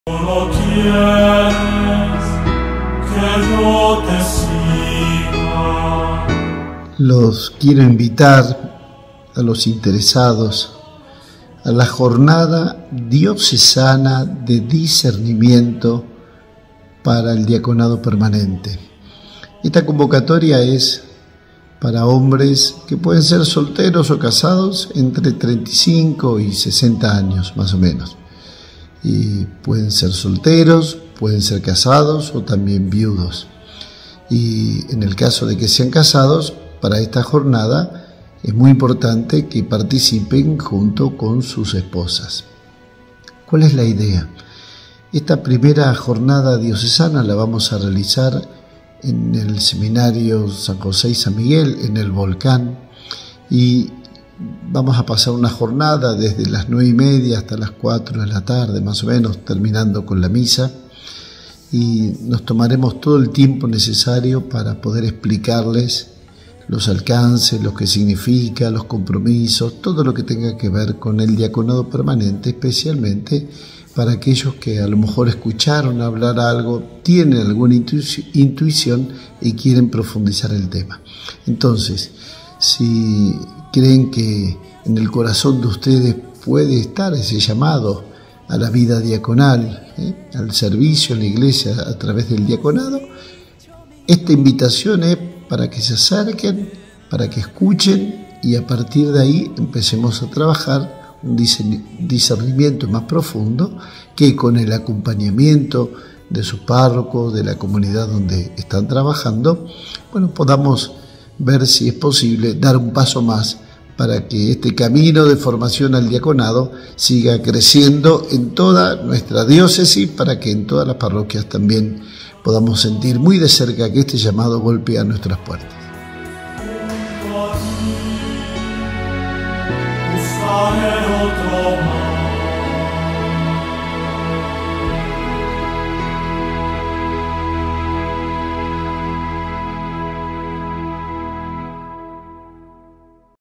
DIACONADO PERMANENTE
Información para participar de la Jornada Diocesana de Discernimiento para el Diaconado Permanente.

En este video, monseñor nos cuenta y nos da información sobre la próxima jornada.